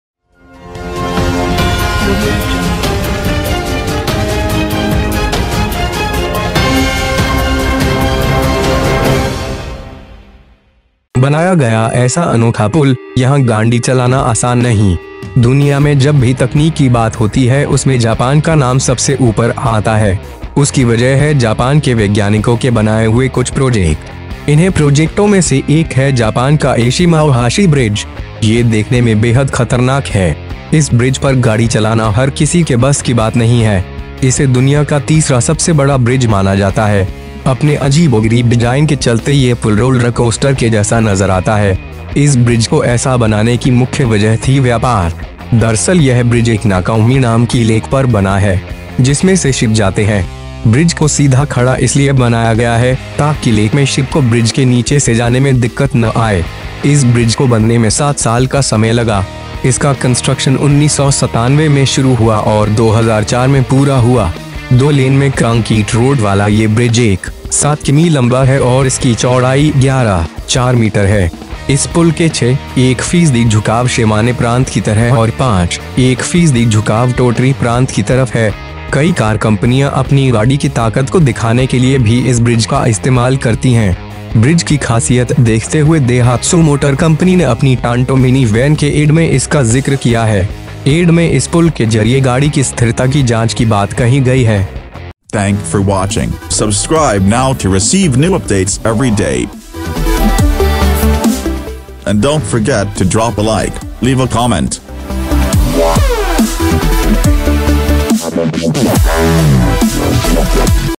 बनाया गया ऐसा अनोखा पुल, यहां गांडी चलाना आसान नहीं, 2 Minute News Today
बनाया गया ऐसा अनोखा पुल, यहां गांडी चलाना आसान नहीं, दुनिया में जब भी तकनीक की बात होती है उसमे जापान का नाम सबसे ऊपर आता है। उसकी वजह है जापान के वैज्ञानिकों के बनाए हुए कुछ प्रोजेक्ट। इन्हें प्रोजेक्टों में से एक है जापान का एशिमा ओहाशी ब्रिज।
Whenever it comes to technology in the world, the name of Japan comes in the top. This is because some of Japan's scientists made some projects. One of these projects is Japan's Eshima Ohashi Bridge.
It is extremely dangerous to see. It's not just about everyone's driving on this bridge. It is considered the world's third largest bridge. Due to its peculiar design, this bridge looks like rollercoaster. The main reason for this bridge was such that the trade This bridge is built on the lake named 'Nakoumi'. From which ship ships. The bridge has been built so that it is not possible to get the ship under the bridge in the lake.
It took seven years to build this bridge, its construction began in 1997, and completed in 2004. This bridge, with a road road in two lanes, is 1.7 km long, and its width is 11.4 meters. This bridge has a 6.1 per cent leaning, like the Shiman province, and 5.1 per cent inclination towards Totri province.
Many car companies also use this bridge to show their vehicle strength.
✔ 2 MINUTE NEWS TODAY: Entertainment News Channel For Everyone
▶ IPL 2017: IPL 10 Schedule, Indian Premier League News, Cricket Match